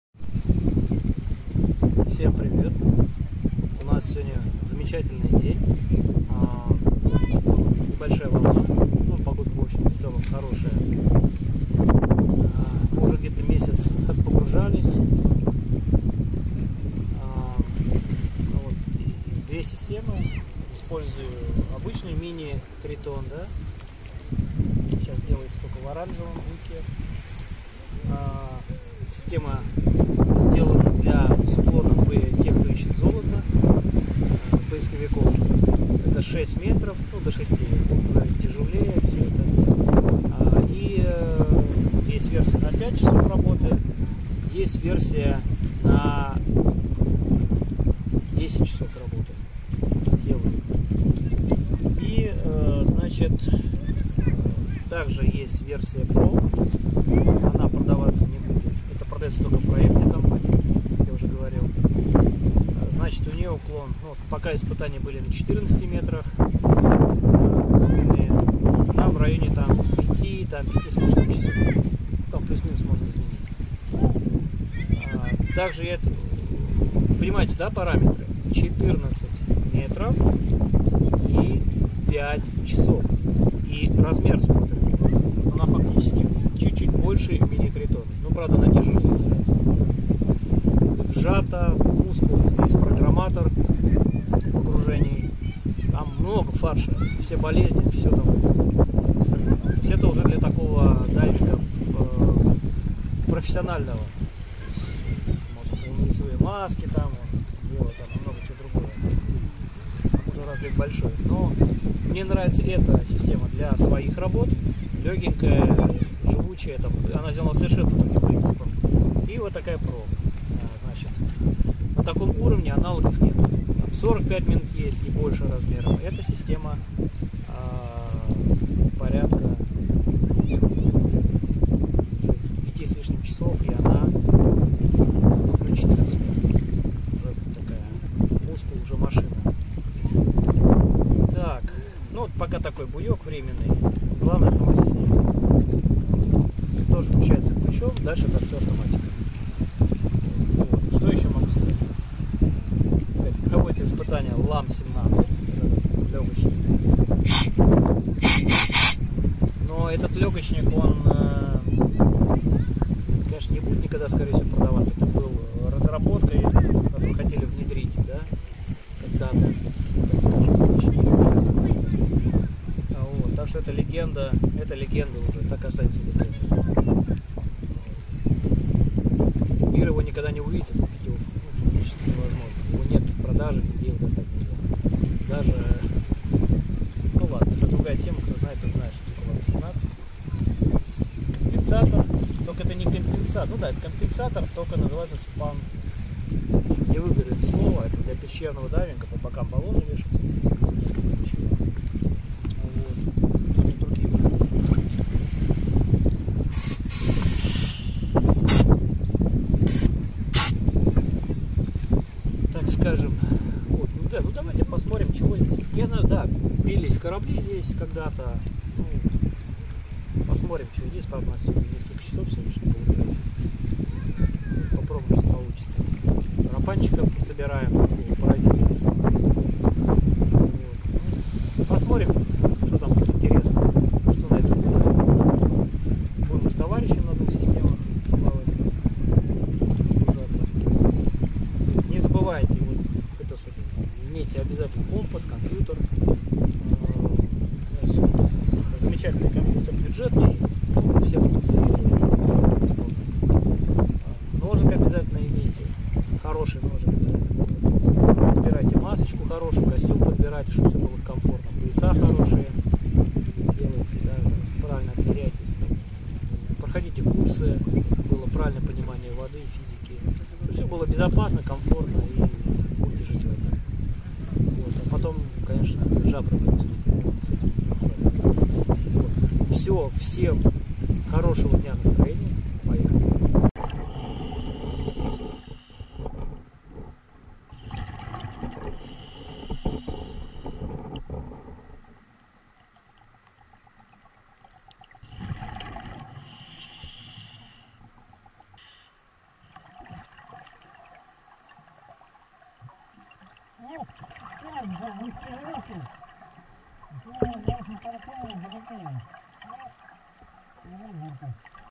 Hookah Diving SystemTriton PRO y mini review, apnea 7 hours at depth without scuba gear
El aparato respiratorio SISTEMA TRITON PRO es el mejor creado hasta la fecha, diseñado para 7 horas de funcionamiento, buceando hasta 14-15 metros, da mucha libertad al buceador, reemplaza 7 botellas de buceo, se carga desde una toma de 220 voltios , un gran recurso de trabajo antes del primer mantenimiento simple, la tecnología está lista para la producción y venta industrial whatsapp telegram  Spain

The SISTEMA TRITON PRO breathing apparatus is the best one created to date, designed for 7 hours of operation, diving up to 14-15 meters, gives a lot of freedom to the diver, replaces 7 scuba tanks, is charged from a 220 volt outlet, a huge resource of work before the first simple maintenance, the technology is ready for industrial production and sale

Дыхательный аппарат SISTEMA TRITON PRO лучшая из созданных на сегодня